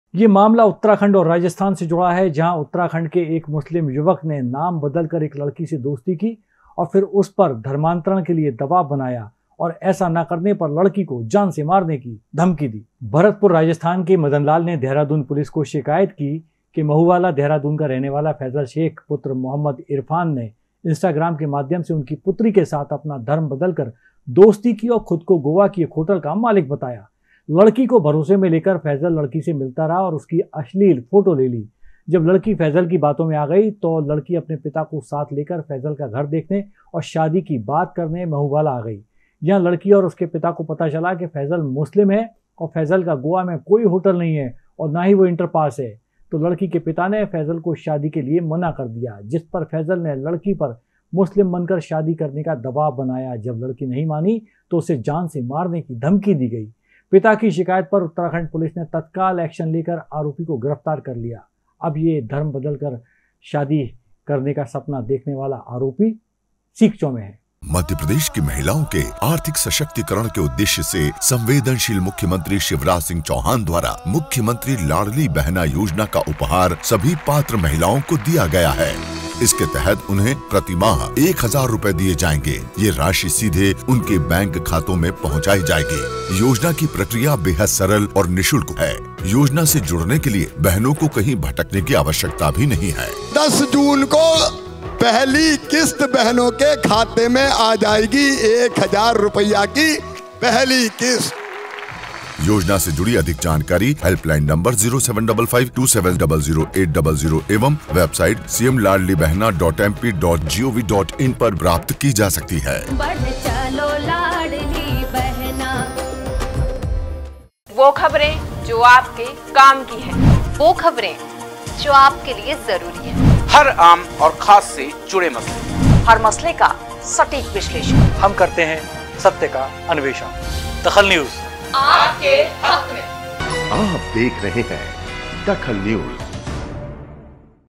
DAKHAL NEWS धर्म बदलकर दोस्ती और धर्मांतरण का दबाव
धर्म बदलकर दोस्ती और धर्मांतरण का दबाव
धर्मांतरण  नहीं करने पर जान लेने की  धमकी